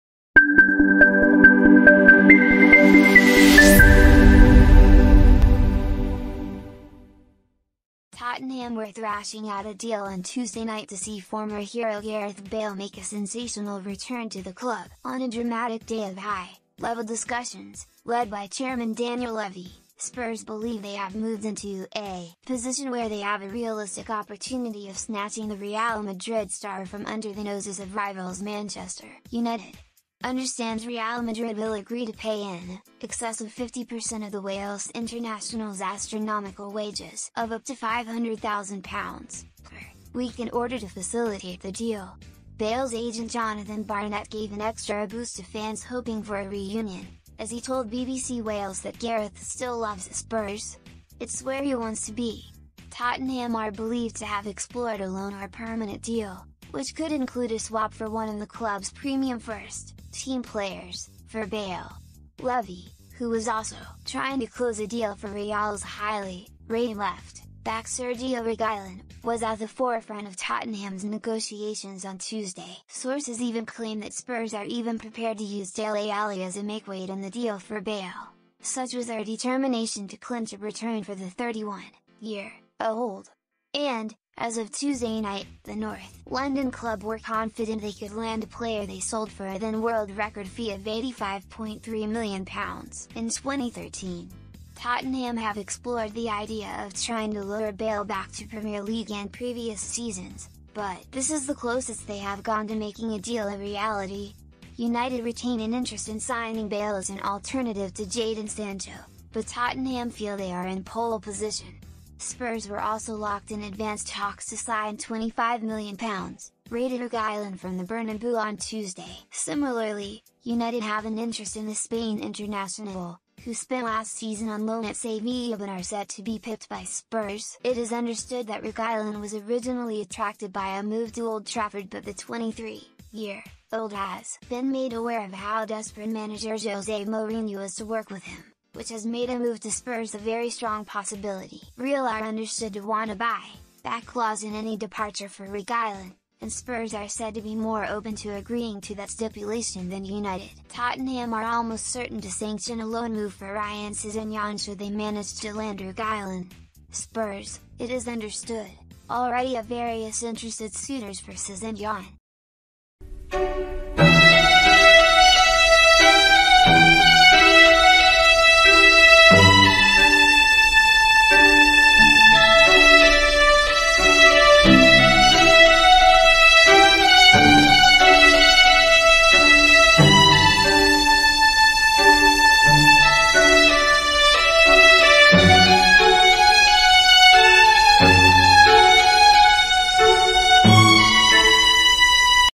Tottenham edge closer to sealing stunning return of Gareth Bale - news today
Tottenham were thrashing out a deal on Tuesday night to see former hero Gareth Bale make a sensational return to the club. On a dramatic day of high-level discussions - led by chairman Daniel Levy - Spurs believe they have moved into a position where they have a realistic opportunity of snatching the Real Madrid star from under the no...